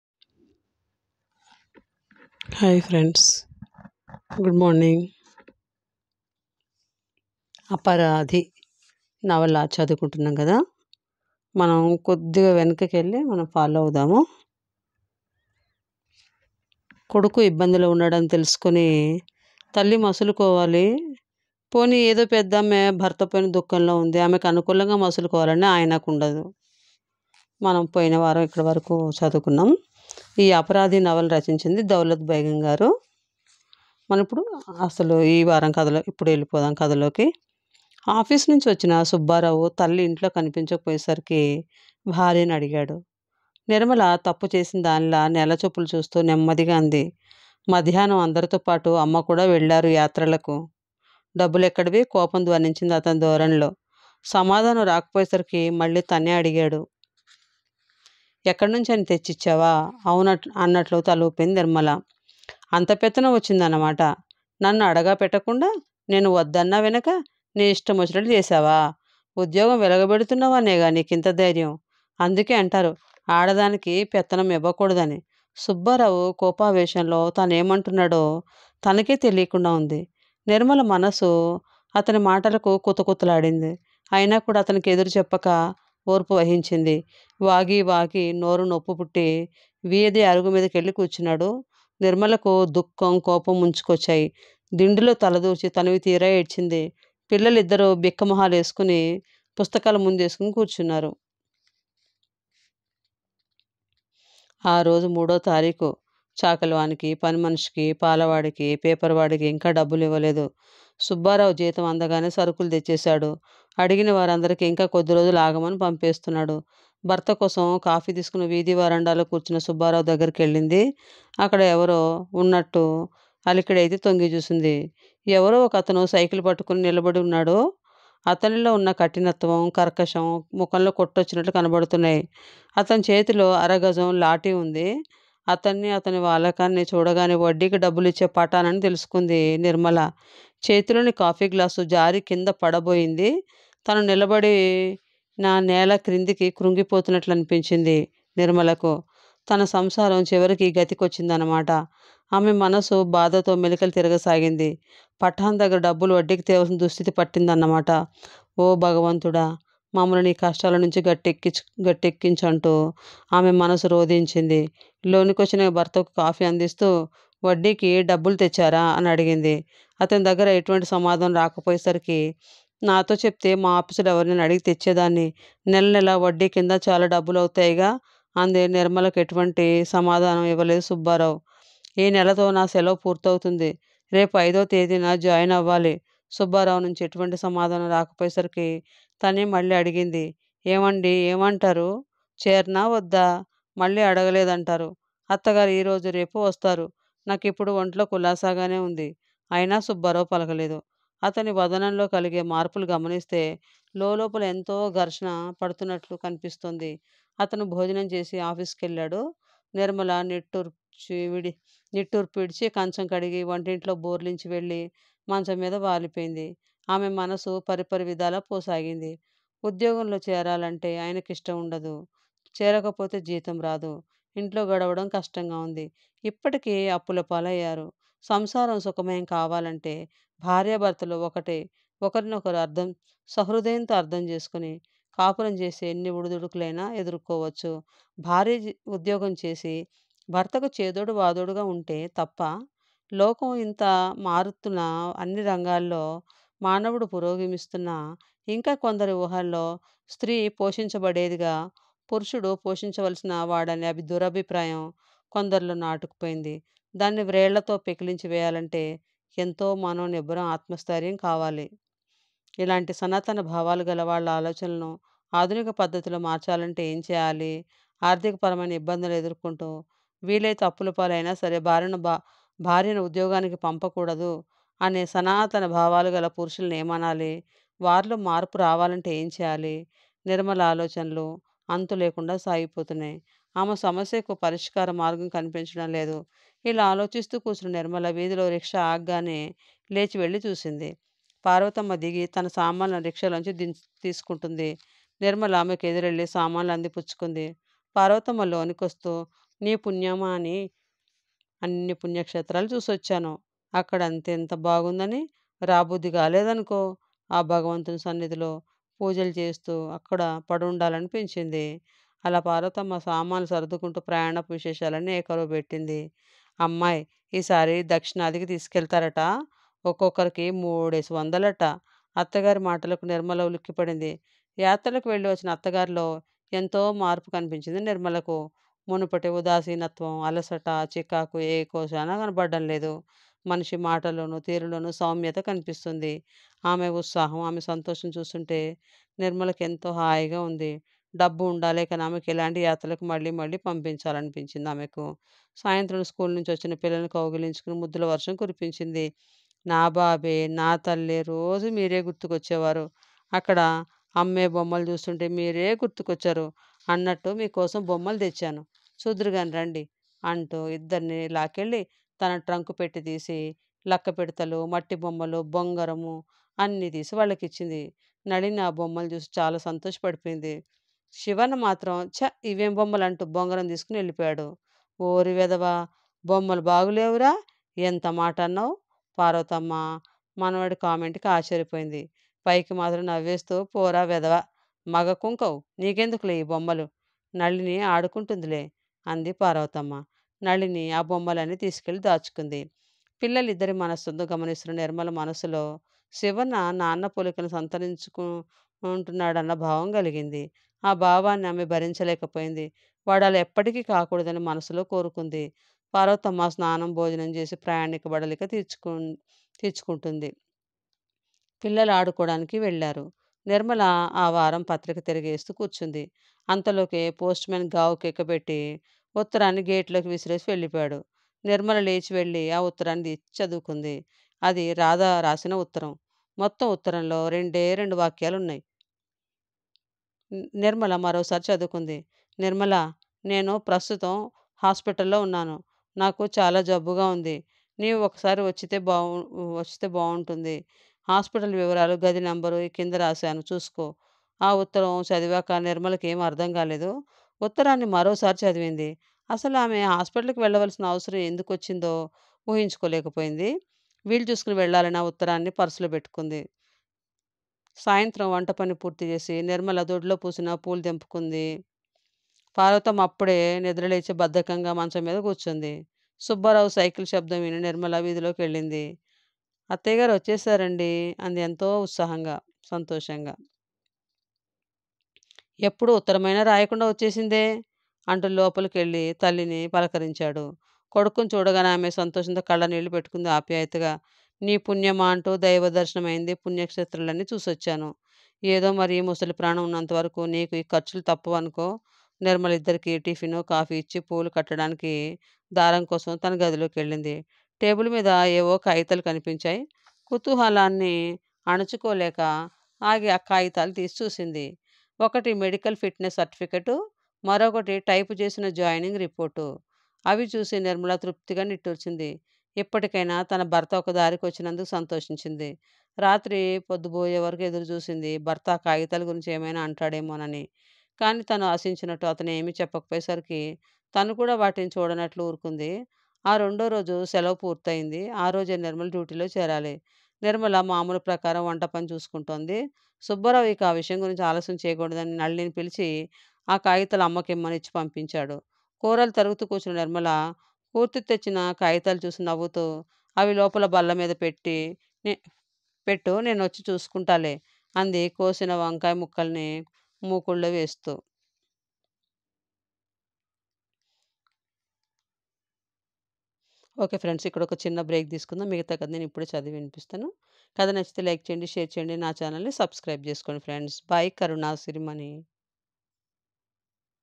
అపరాధి నవల పార్ట్ -9 aparaadhi novel part -8 దౌలత్ బేగం పఠాన్ అత్తగారు తీర్థ యాత్రలు
అపరాధి నవల పార్ట్ -9 aparaadhi novel part -9
దౌలత్ బేగం గారు రచించినది. వినండి karunasirimani akira advait baby boy funny videos and photos 😂😂🤣🤣😂😅🤣🤣😂🤣
@Akira Advait baby boy funny videos and photos 😂😂🤣😅😂😅🤣😅
ప్రస్తుత కథలో  నిర్మలా అత్తగారు యాత్రలకు వెళ్లడానికి డబ్బు పద్మజ అది తీసు కుని అత్త గారి యాత్రలకు వెళ్లడానికి ఇస్తుంది. సుబ్బారావు ఆ ఇషయం లో నానా మాటలు తిట్టి పోస్తాడు.  నిర్మల చాలా బాధ పడుతుంది.
ఒక రోజు పఠాన్ డబ్బు కోసం ఇంటికి వస్తాడు. వడ్డీకి డబ్బులు తెచ్చారా అని అడుగు తుంది కానీ సుబ్బారావు సమాధానం చెప్పడు. తరువాత జాబ్ చేస్తానని అధు తుంది దానికి సమా దానం చెప్పడు. .ఒక రోజు ఒక ఉత్తరం వస్తుంది. ఆ letter లో తనకి ఆరోగ్యం బాగా లేదనీ చాలా జబ్బు చేసిందని హాస్పిటల్ పేరు .. రూం నంబర్ వివరాలు రాడి ఉన్నాయి. మిగతా కథ వినండి